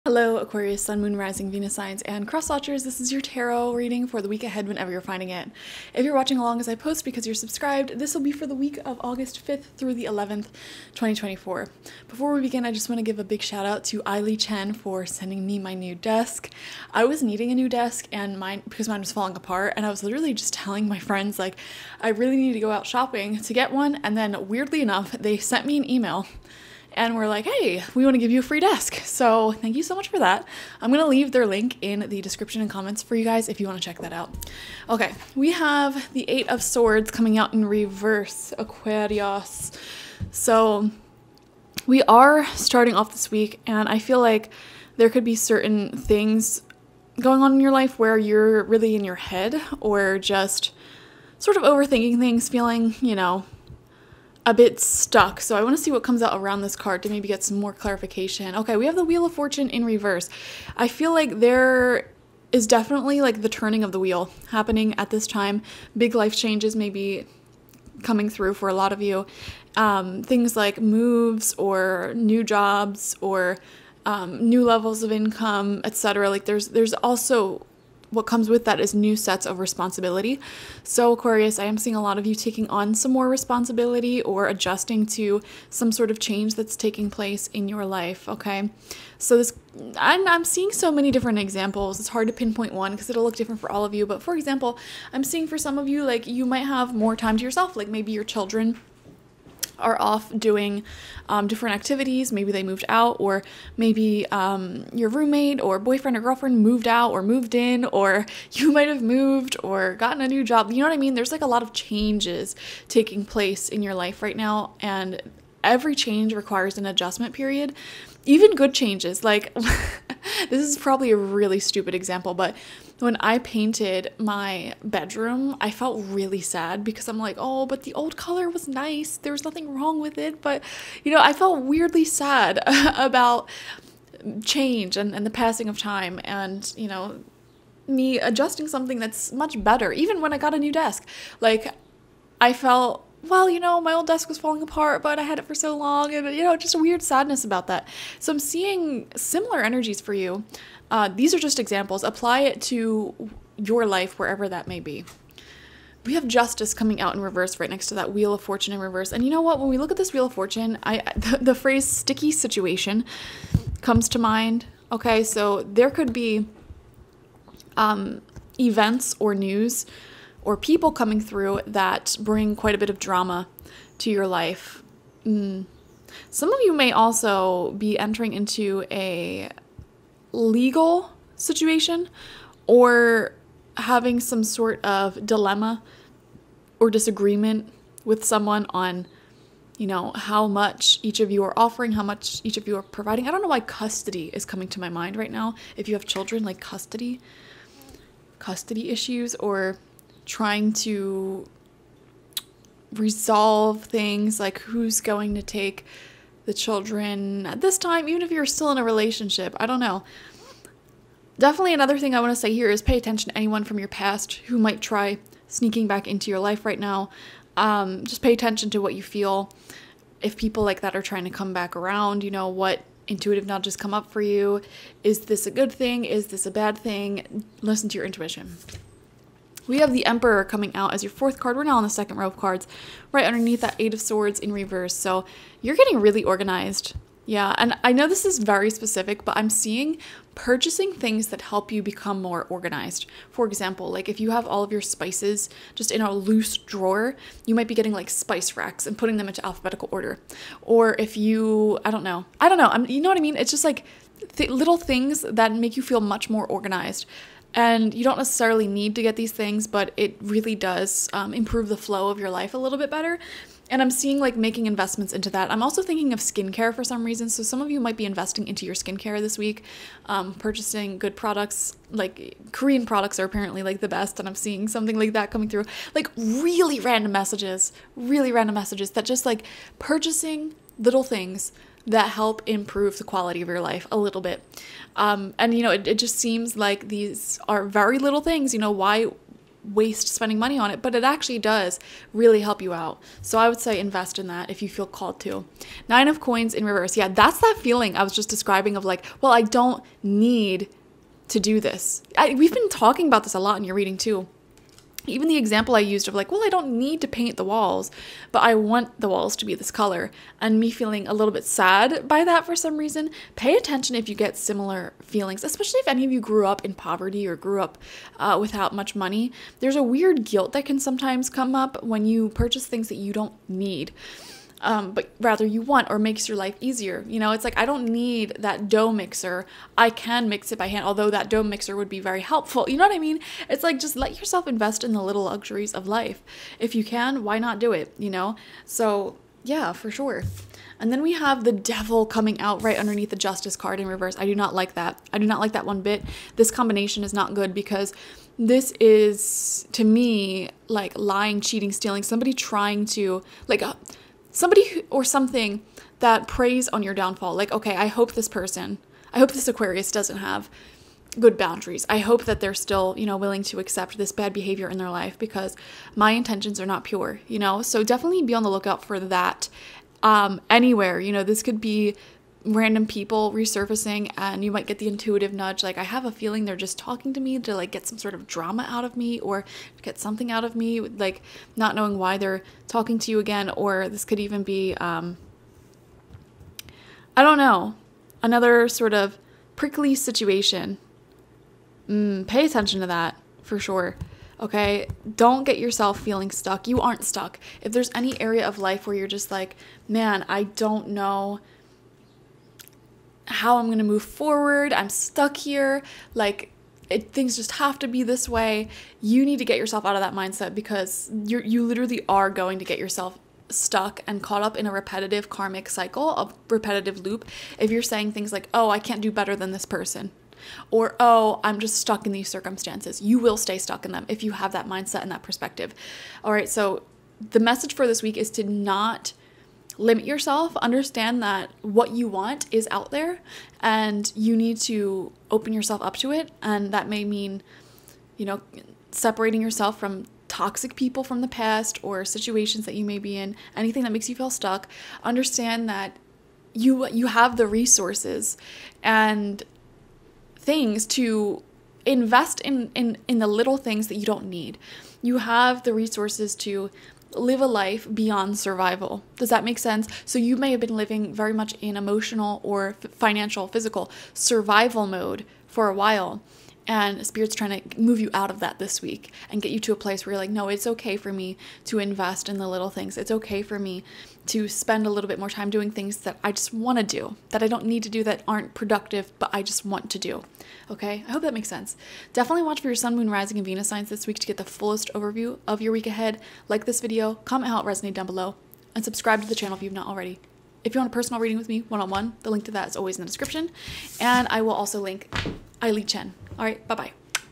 AQUARIUS ♒ Be Careful Of Limiting Beliefs! Tarot Reading
Welcome, Aquarius! This is your tarot reading for the week of August 5th to the 11th, 2024 or whenever you are finding this.
Watch for your sun, moon, and rising signs.